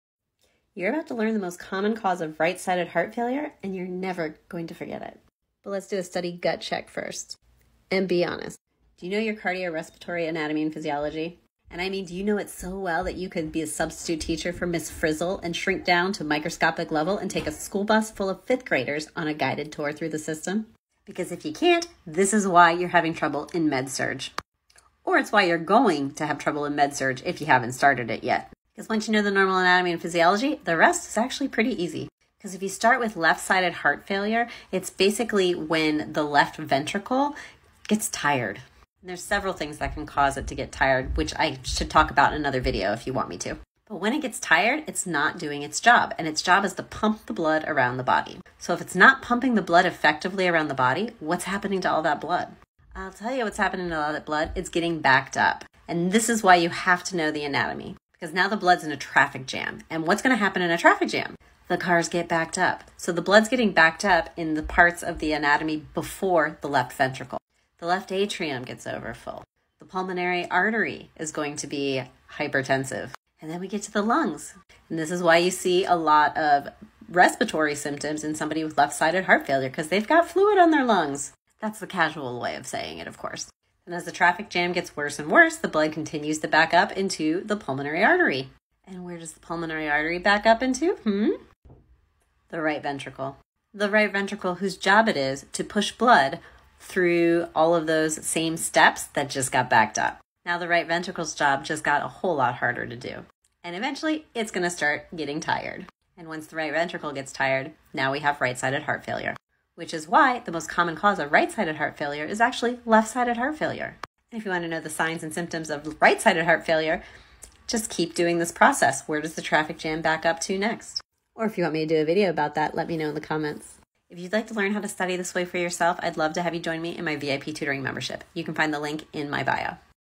Easiest Way to Finally Understand Heart Failure
If you’ve ever memorized the symptoms of heart failure and still felt lost on exams, here’s why 👇

You don’t need another flashcard—you need to understand the traffic jam. 🚗💨

When the left ventricle gets tired, blood backs up—first in the lungs (causing fluid buildup), and then to the right ventricle, which now has to push against that backup.
Eventually the right side gets tired too.

That’s why the most common cause of right-sided heart failure is left-sided heart failure.